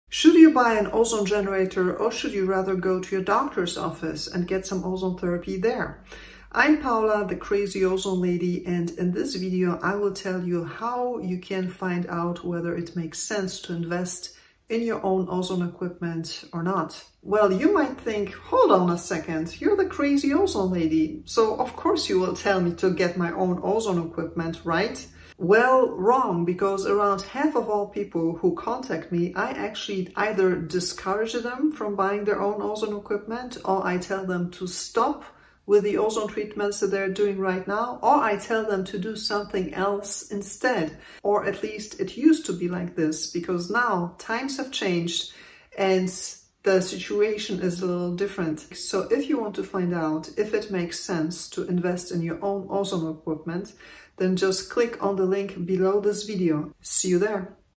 Should You Buy Your Own Ozone Generator? (For Ozone Therapy at Home)
How to figure out if you should buy your own ozone generator.